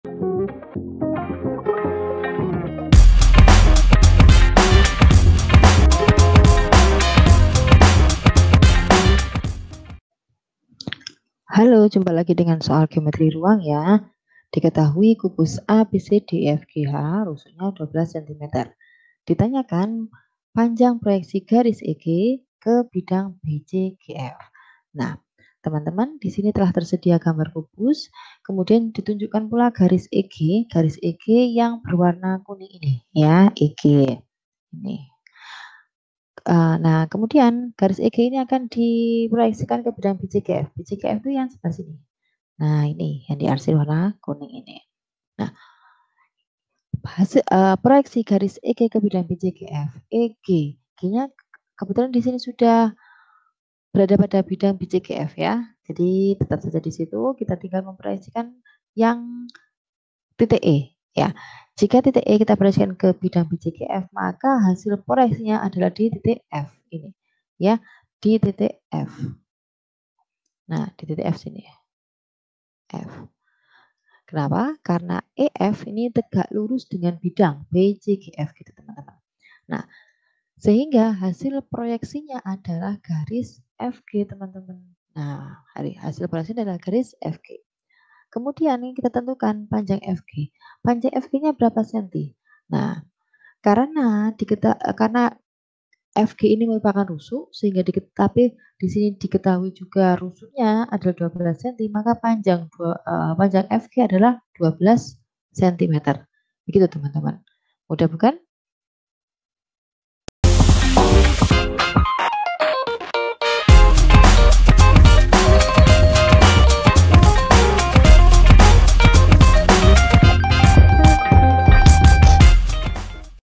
Diketahui kubus ABCD.EFGH dengan panjang r... | Matematika Untu | Kelas 12 | 1
✅ Dalam video ini kita akan membahas: Diketahui kubus ABCD.EFGH dengan panjang rusuk 12 cm.

Tentukan :
b) proyeksi garis EG ke bidang BCGF dan hitung panjang proyeksi tersebut.
Tema:
Kelas: 12
Mata Pelajaran: Matematika
Bab: 1
Buku: Matematika Untuk SMA/MA Kelas XII Kelompok Wajib

👉 Buku yang telah tersedia:
{PLAYLIST_LINK}
Di mana kamu dapat menemukan kami?